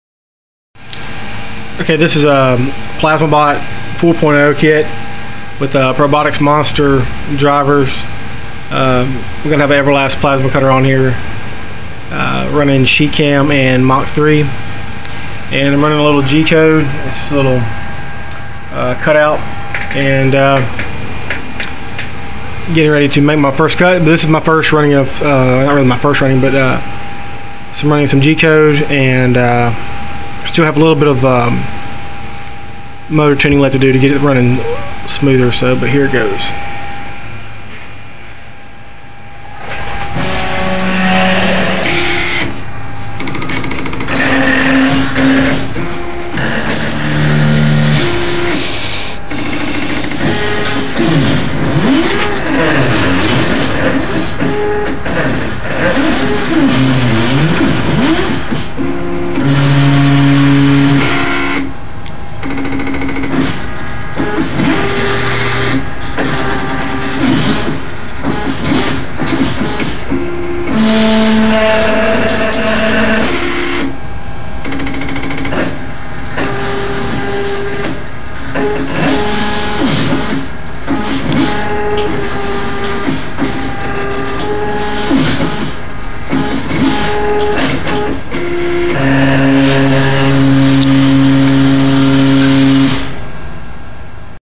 Plasmabot 4.0 CNC Plasma Cutter Part 2
Plasmabot 4.0 CNC gantry kit from No Sleep Studio with a Probotix Monster 4 axis CNC motors and electronics.  I will be using an Everlast PP60C plasma cutter.